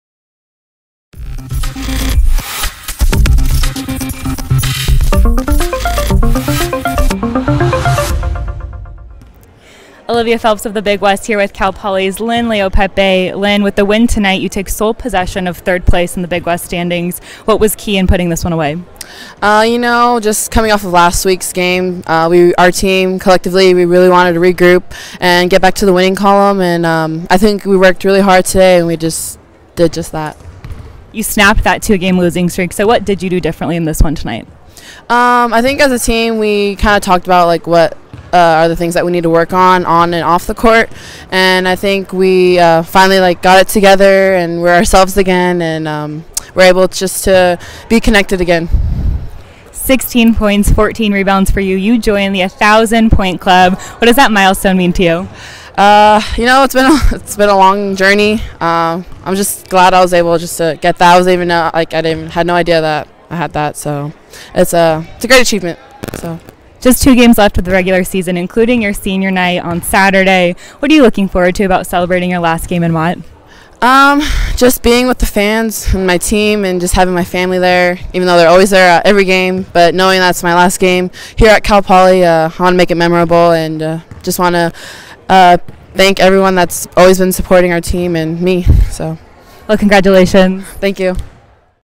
WBB: Dynn Leaupepe (CP) After Joining The 1000-Pt Club
Cal Poly defeated UC Santa Barbara 73-72 in a midweek BlueGreen matchup with help from senior Lynn Leaupepe who notched 16 points and 14 rebounds. The senior joined the 1,000-point club in the victory.